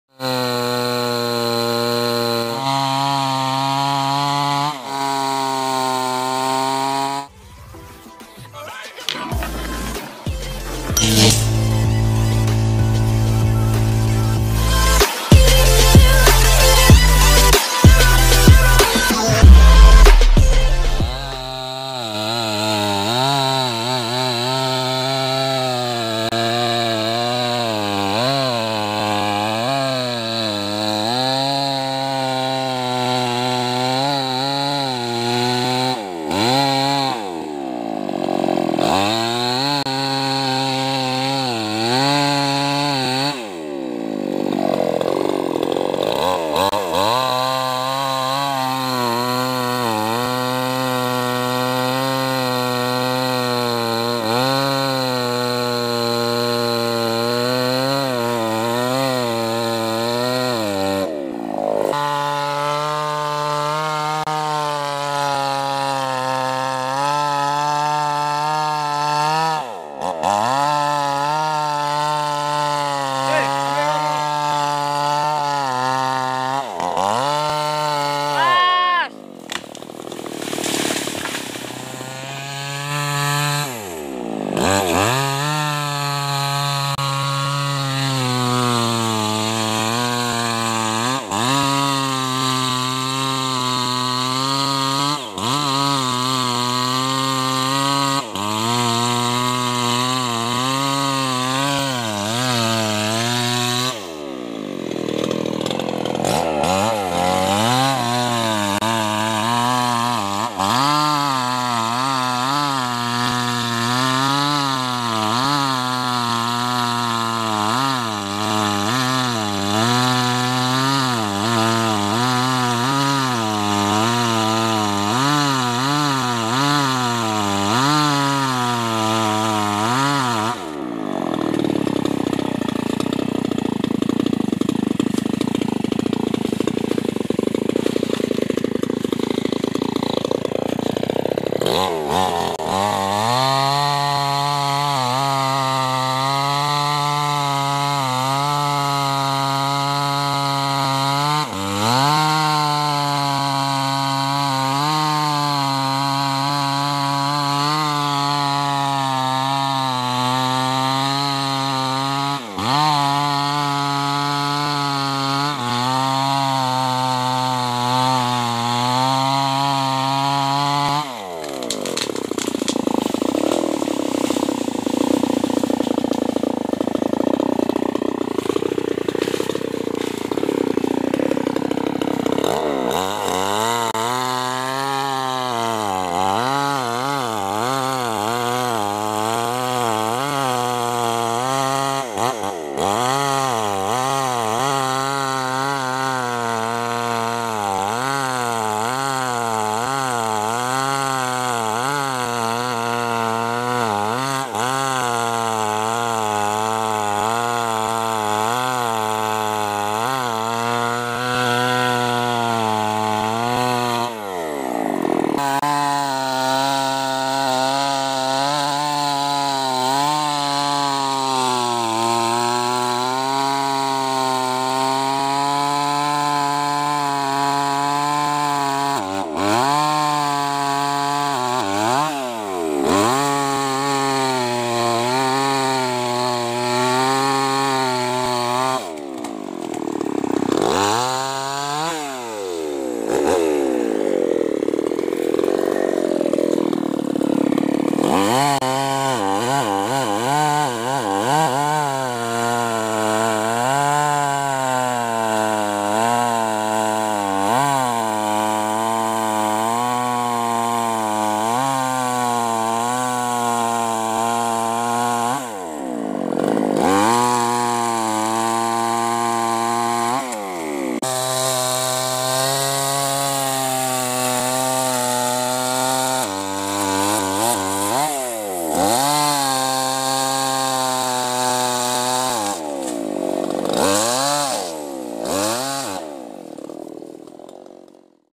juar dan akasia kayu keras dan berat ditebang dengan gergaji mesin new west 588 tune up
Tebang 2 pohon keras berat juar dan akasia di tebang dengan gergaji mesin new west 588 tune up di kebun warga